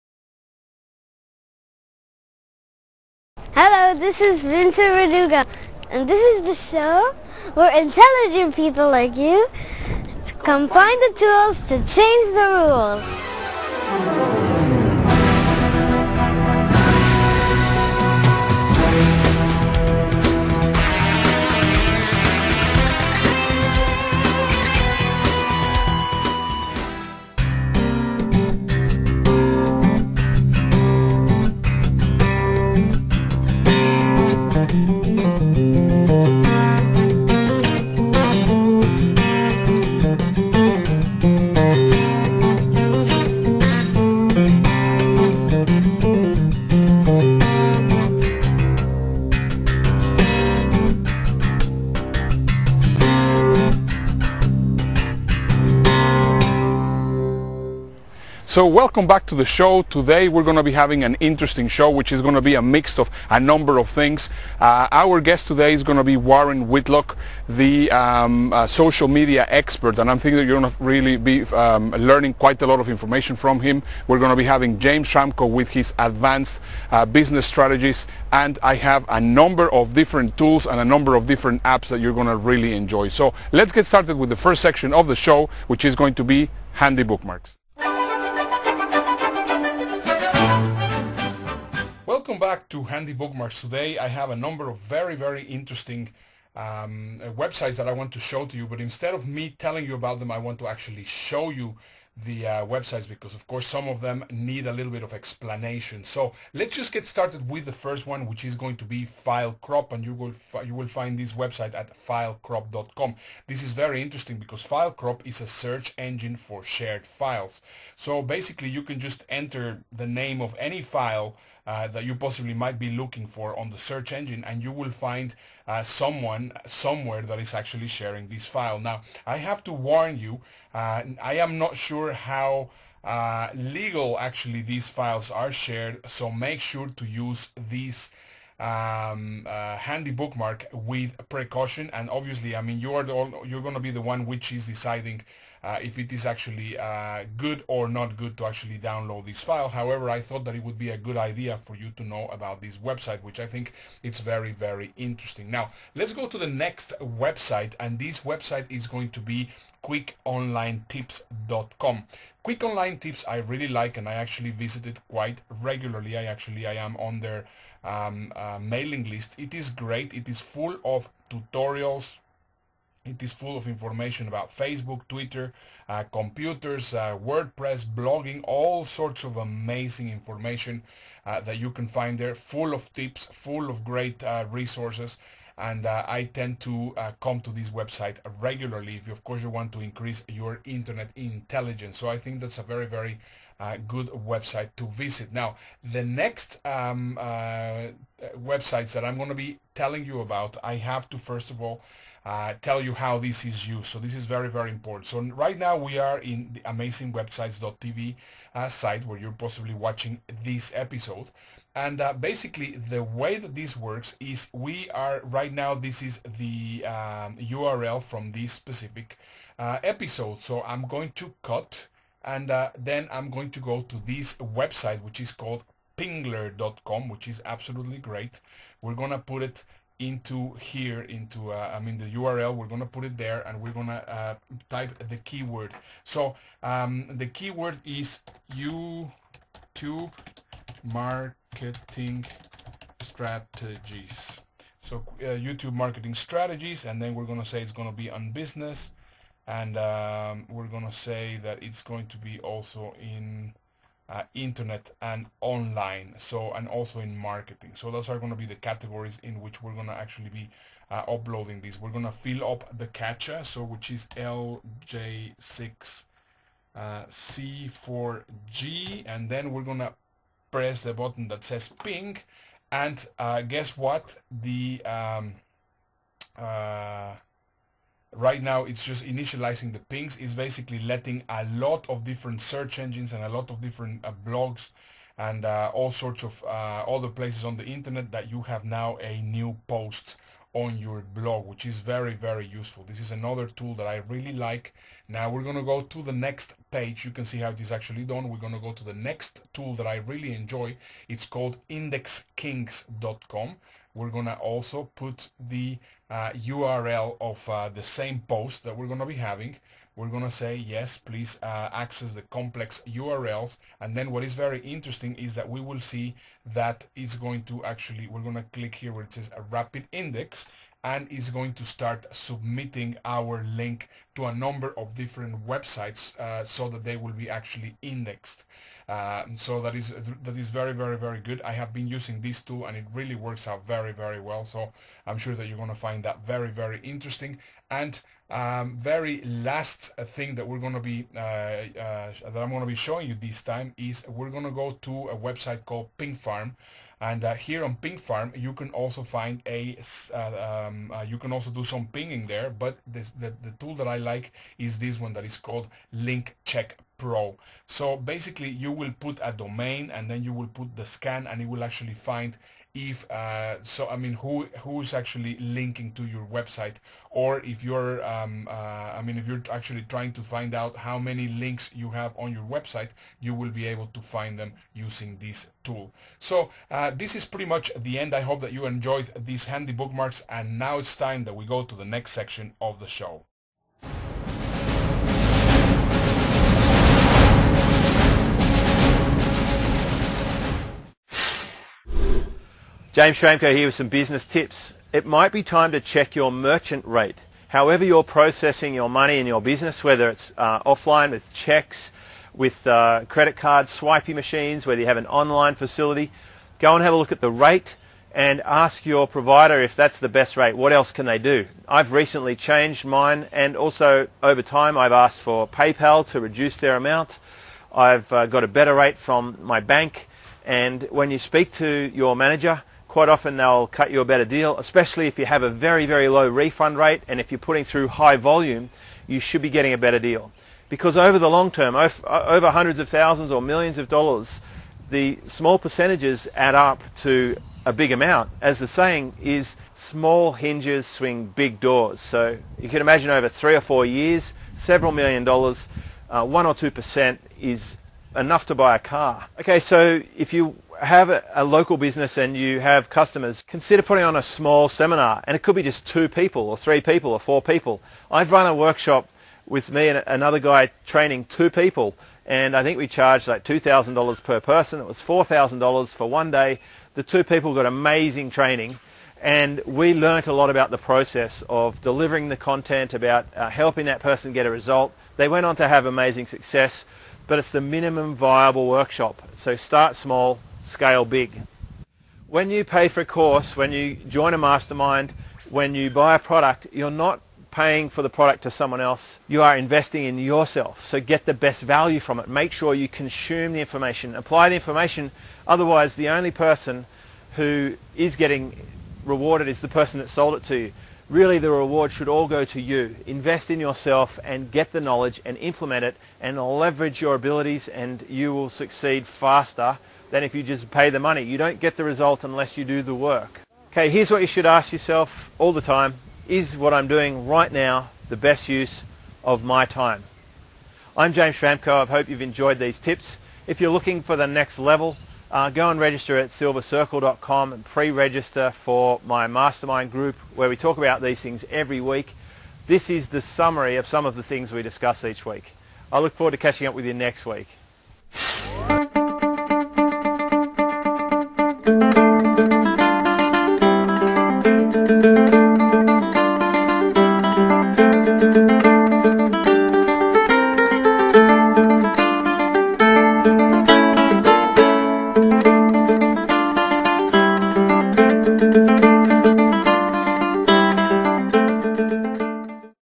Internet Secrets Revealed
Internet Secrets revealed, here are 5 really useful websites for you learn. Internet Secrets that nobody shares with you on the latest episode of Amazing Websites Television with Ernesto Verdugo